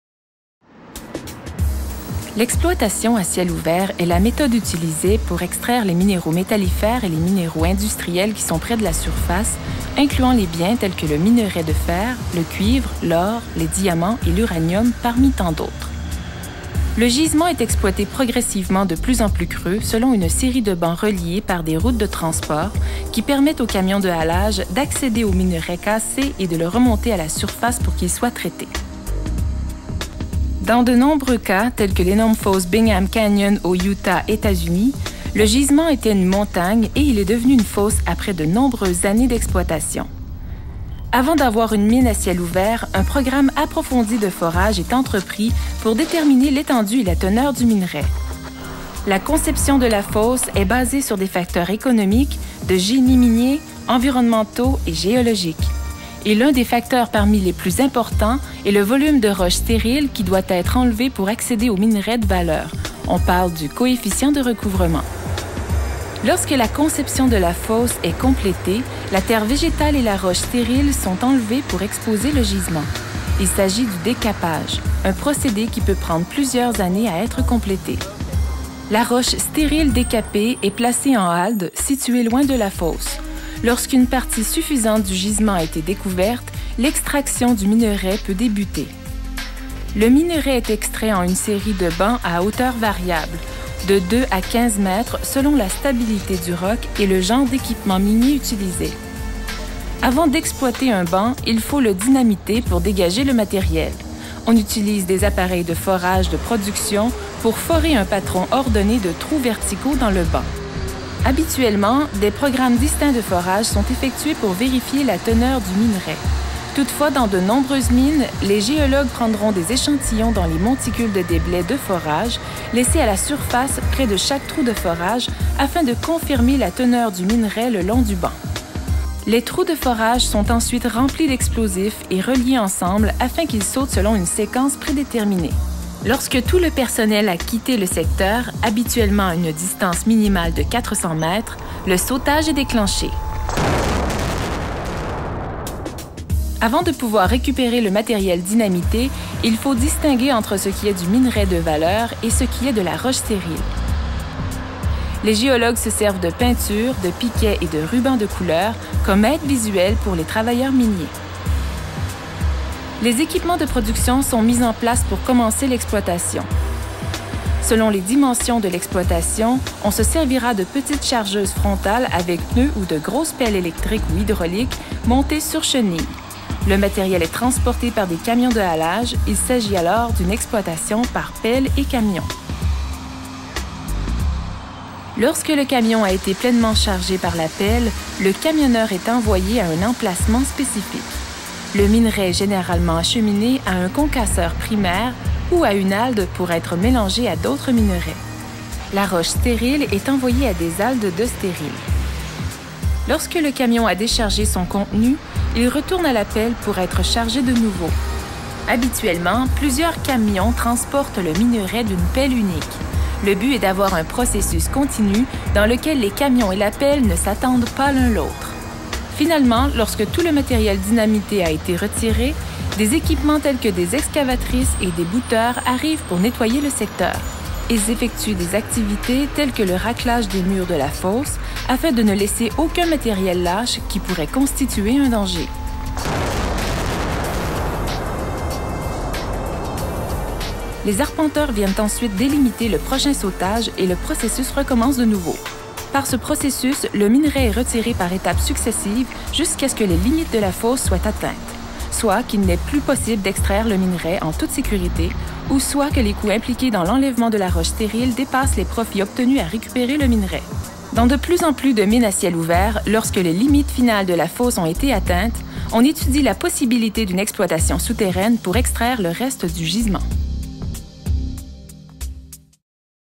L'exploitation minière à ciel ouvert
Vidéo explicative sur le procédé d'exploitation minière à ciel ouvert produit au Québec